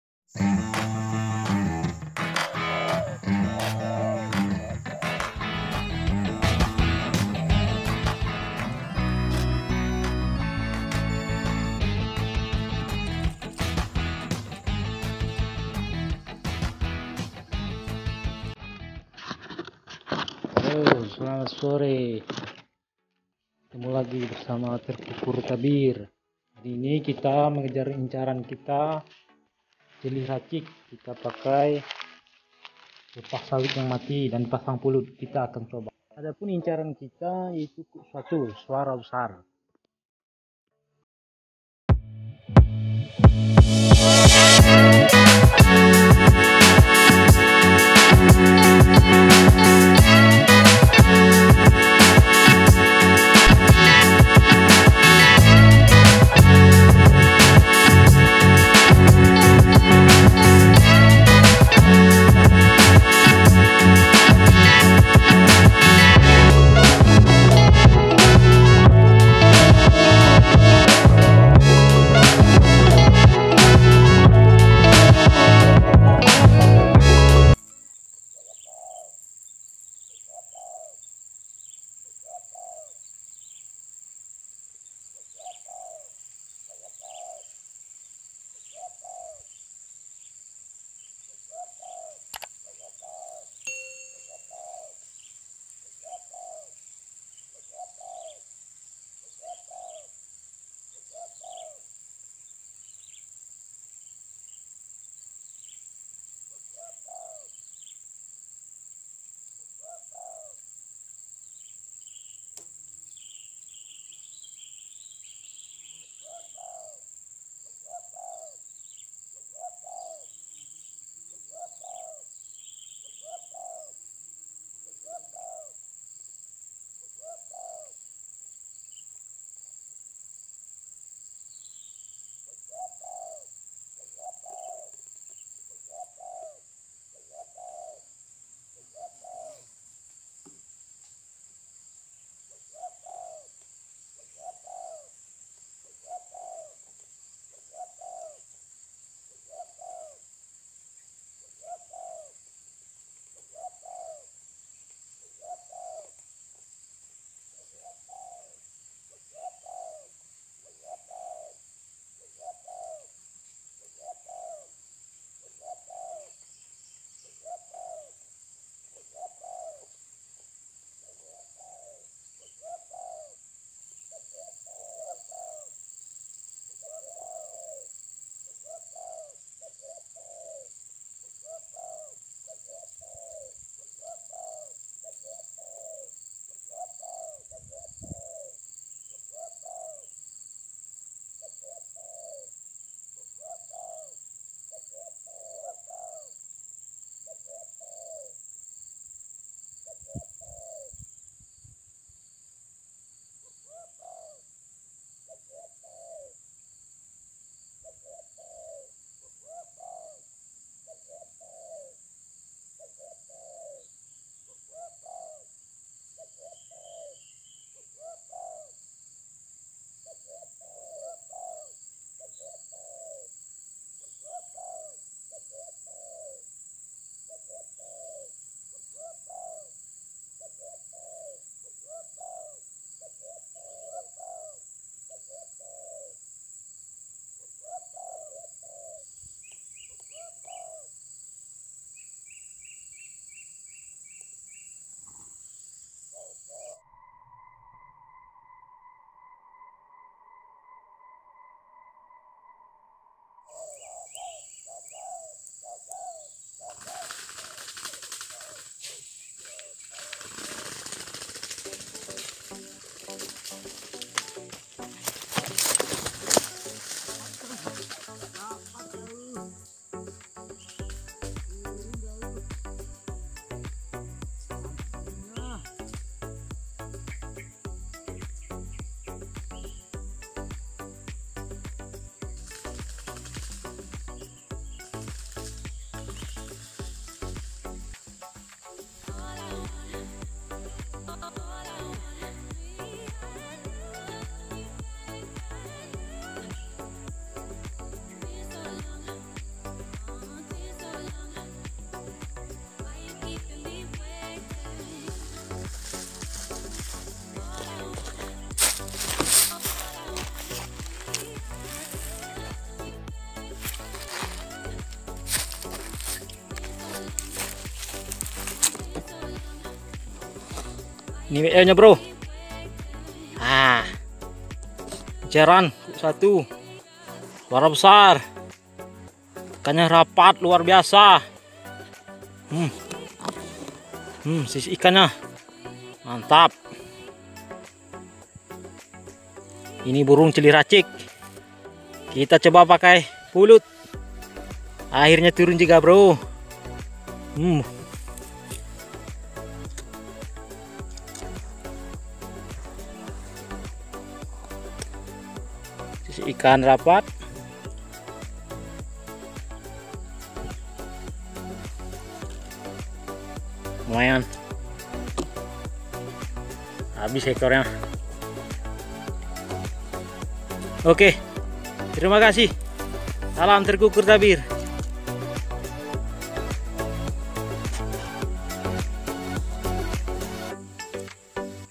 Kita gunakan pelepah sawit mati demi incaran suara idaman pemikat ~TERKUKUR TABIR
sore ini kita kejar incaran, karena incaran celir racik jadi kita ngunakan alat bantu pelepah sawit